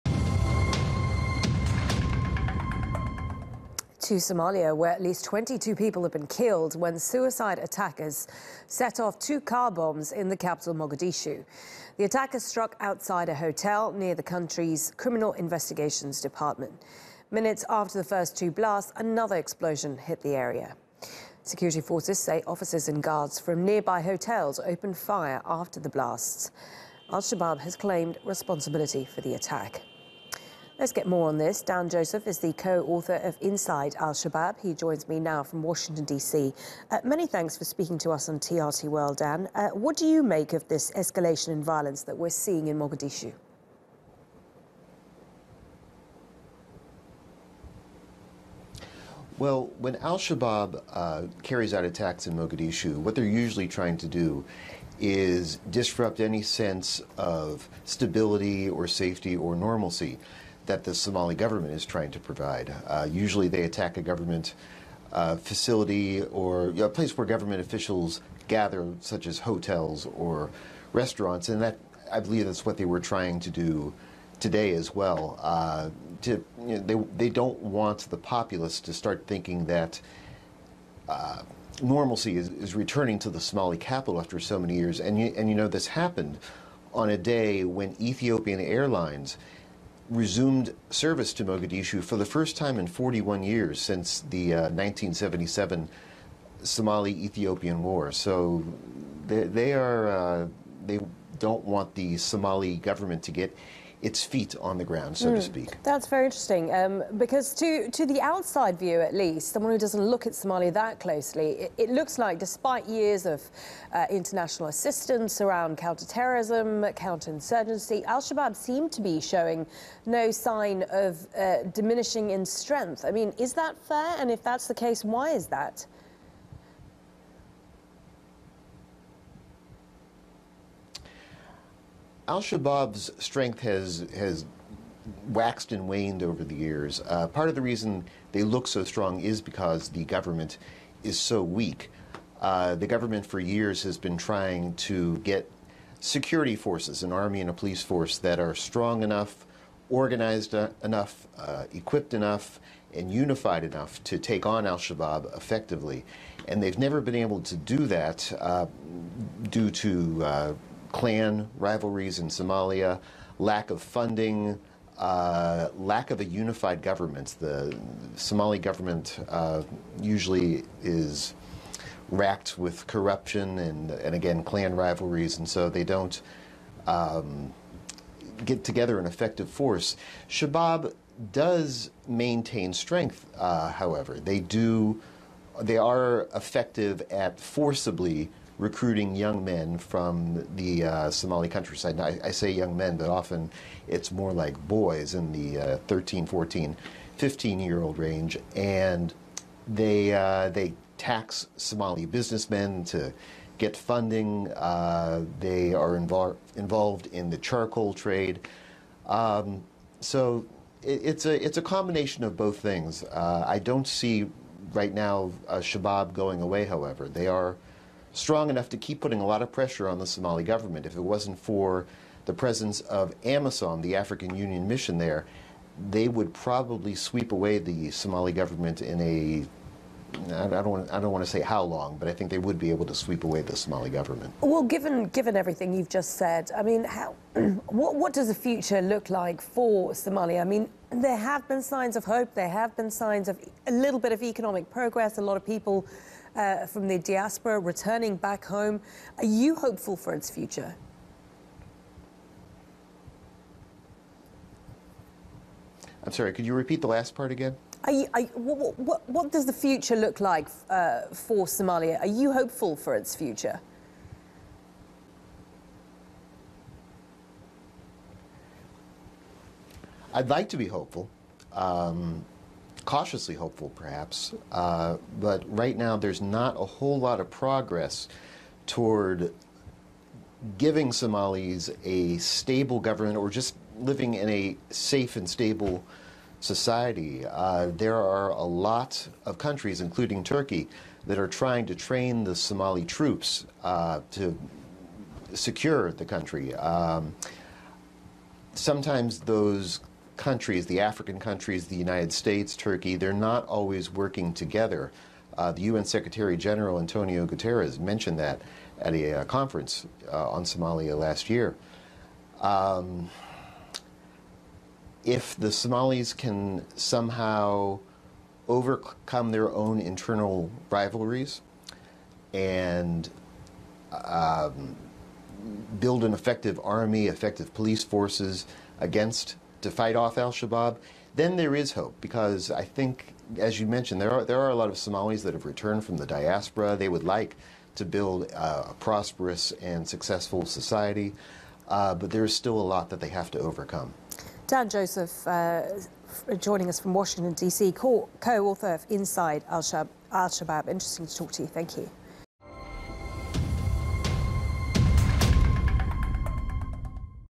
Suicide attackers target hotel in Somalia
Co-author of "Inside Al-Shabaab" Dan Joseph talks to TRT World about the power of the insurgent group and the future of Somalia.